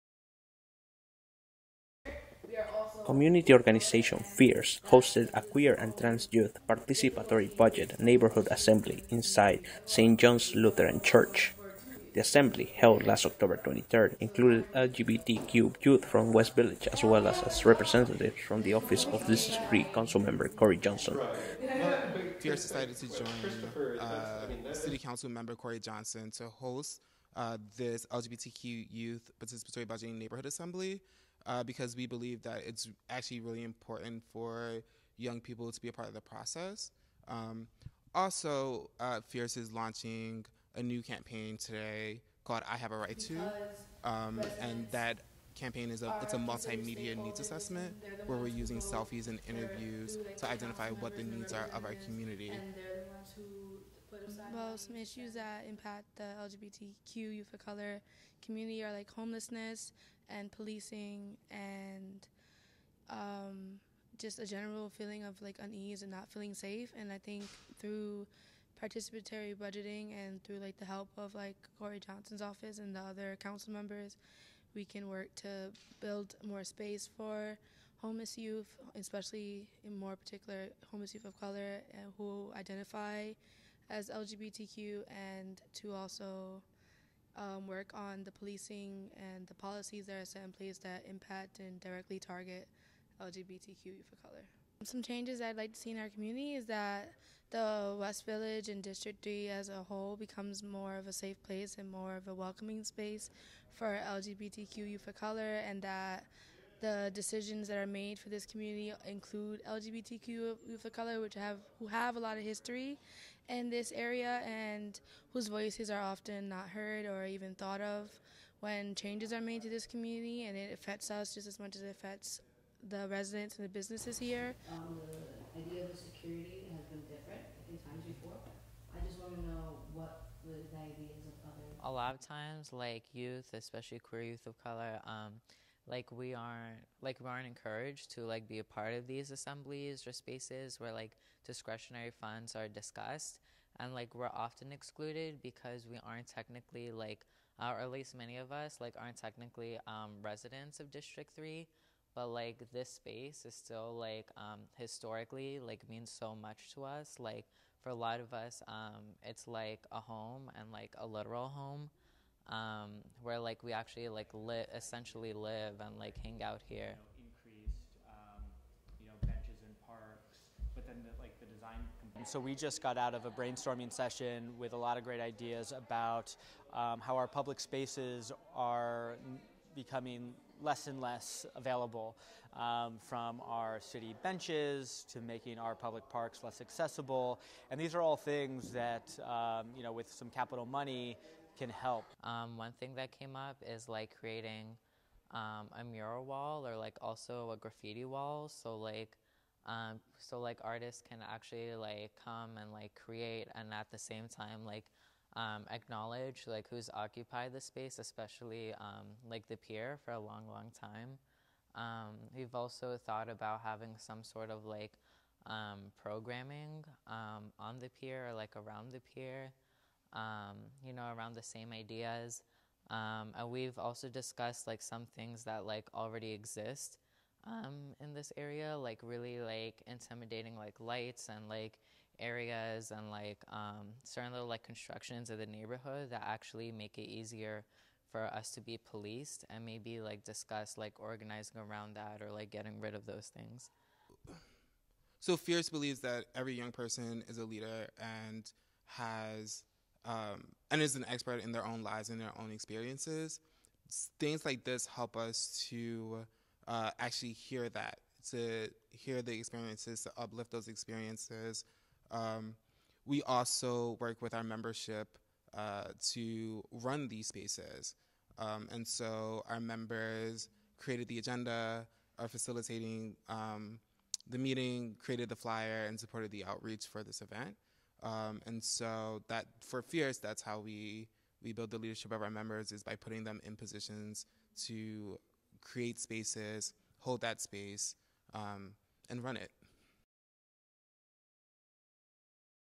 iHaveARightTo FIERCE
Participatory Budget is a process in which residents of a given area can contribute ideas as to how to dispense capital funds.

Special Thanks To:
Fierce NYC

Produced By:
Percy Lujan